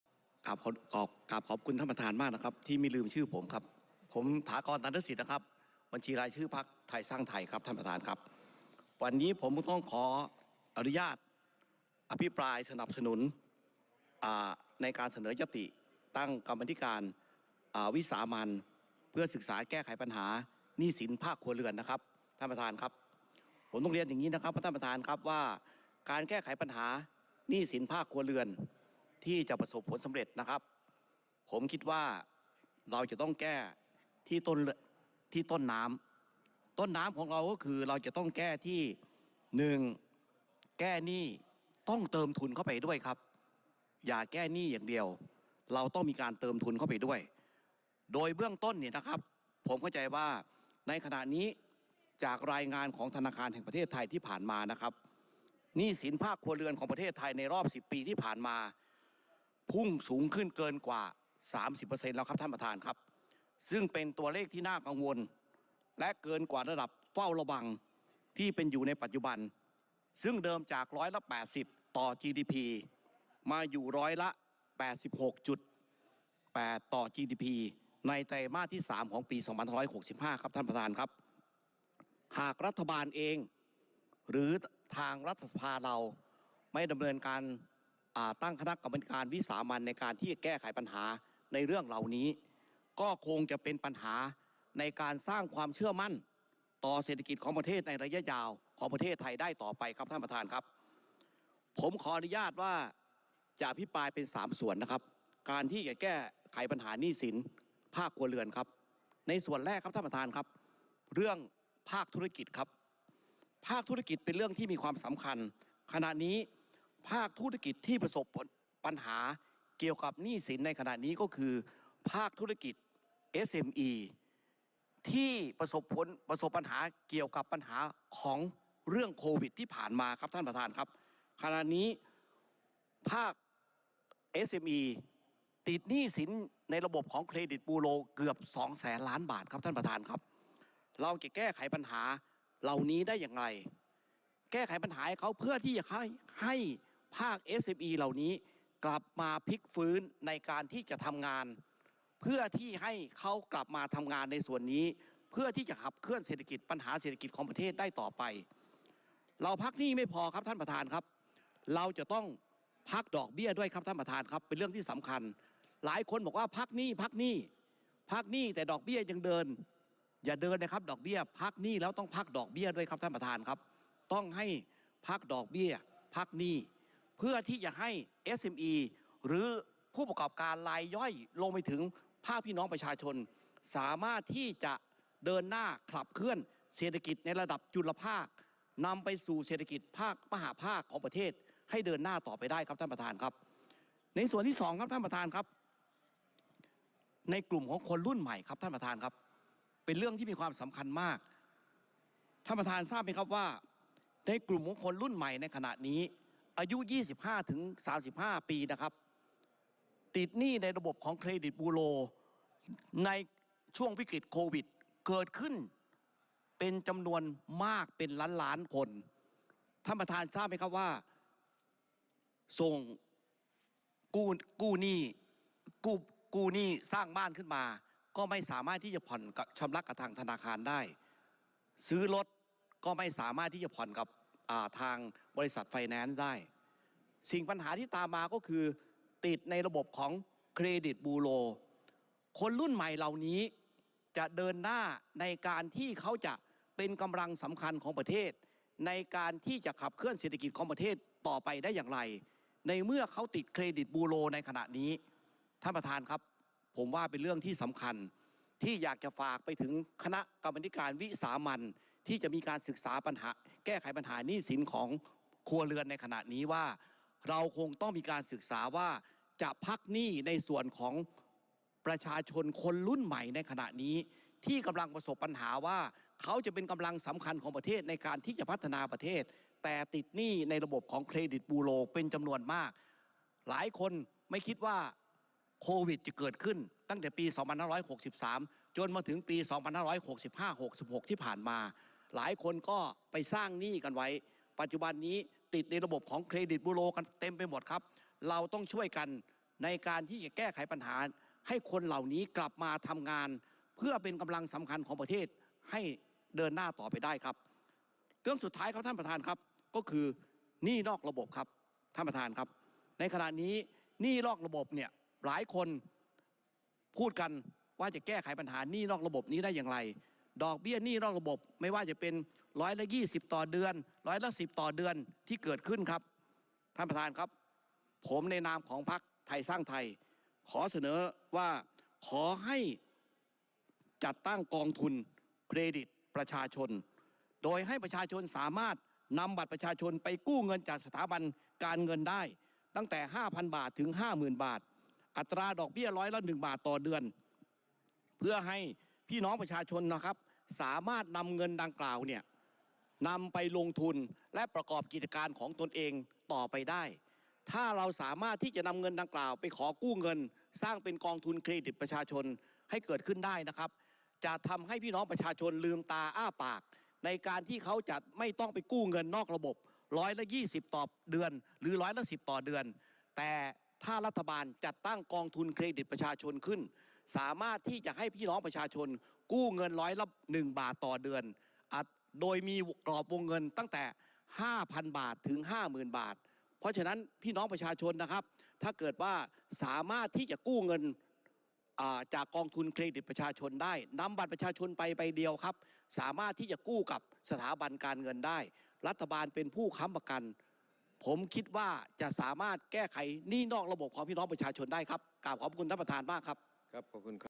ฐากร ตันฑสิทธื์
🔴ญัตติ..ขอให้สภาผู้แทนราษฎร ตั้งคณะกรรมาธิการวิสามัญ
พิจารณาศึกษาหาทางแก้ไขปัญหาหนี้สินครัวเรือน

นายฐากร ตัณฑสิทธฺิ์

การประชุมสภาผู้แทนราษฎรชุดที่ 26 ปีที่ 1 ครั้งที่  24-25 (เป็นพิเศษ)
 (สมัยสามัญประจำปีครั้งที่หนึ่ง)
วัน พฤหัสบดี-ศุกร์ ที่ 19-20 พ.ศ. 2566

     - ญัตติ เรื่อง  ขอให้สภาผู้แทนราษฎรตั้งคณะกรรมาธิการวิสามัญพิจารณาศึกษาแก้ไขปัญหาหนี้สินครัวเรือน

ครั้งที่25 เป็นพิเศษ